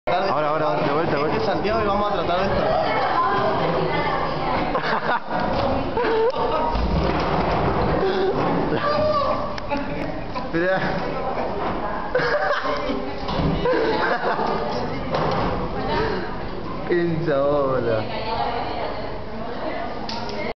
Dino despierta a Santi
Una forma muy particular de despertar a alguien luego de una noche de serenata...